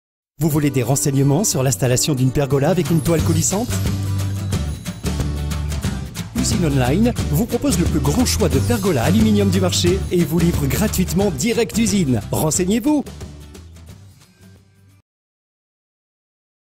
installation pergola toile coulissante
Vous voulez des renseignements sur l’installation d’une pergola avec une toile coulissante ? Usine-online vous propose le plus grand choix de pergolas aluminium du marché et vous livre gratuitement direct usine ! Renseignez-vous !!